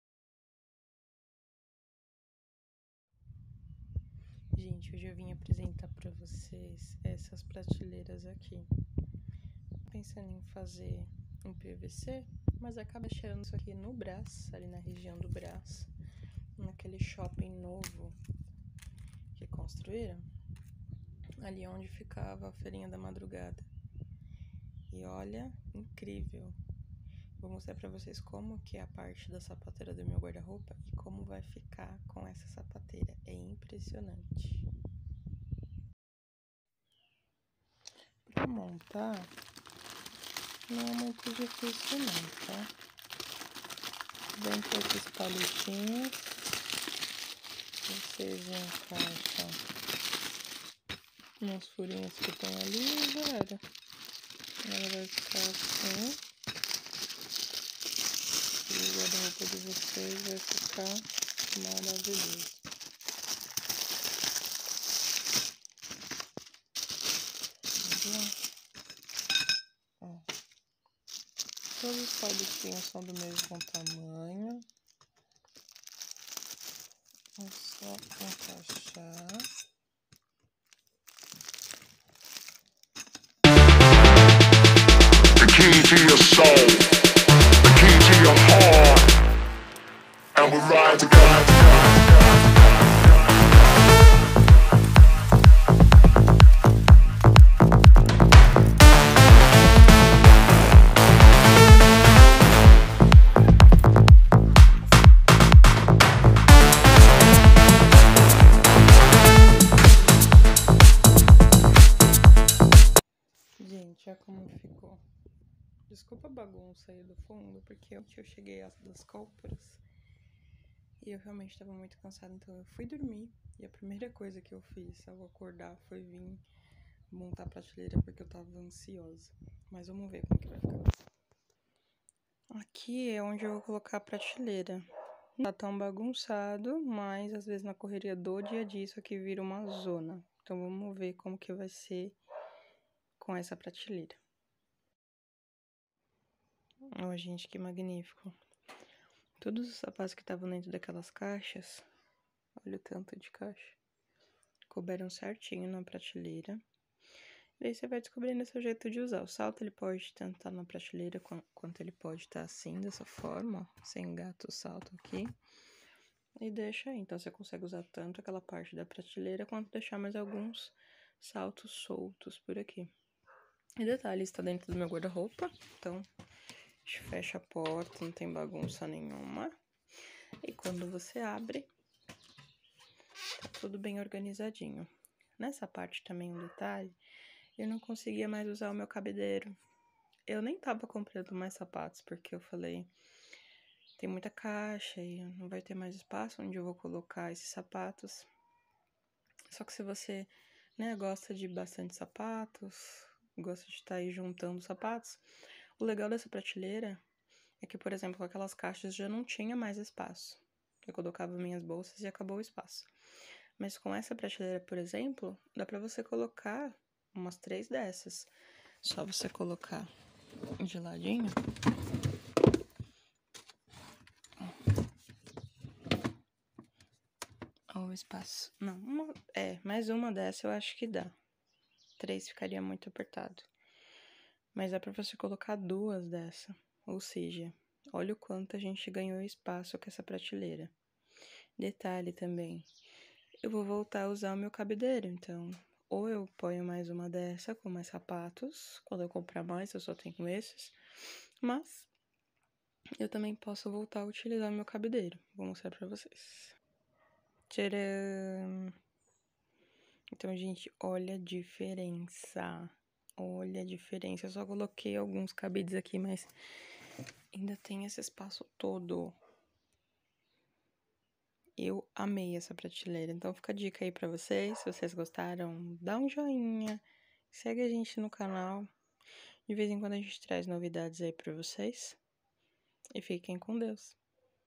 otimização de espaço no guarda roupa.
prateleira otimizadora de espaço para organizar guarda roupa.